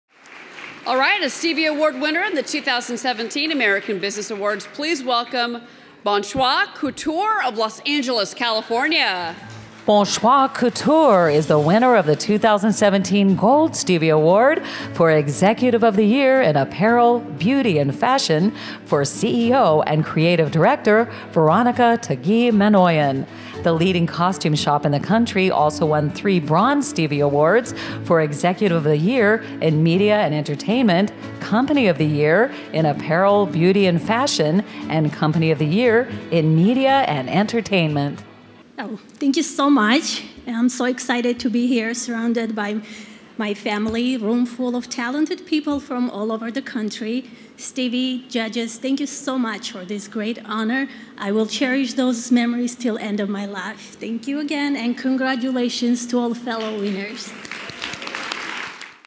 Bon Choix Couture Wins Stevie Award in 2017 American Business Awards
The 15th Annual American Business Awards were presented on Tuesday, June 20 at the Marriott Marquis Hotel in New York City.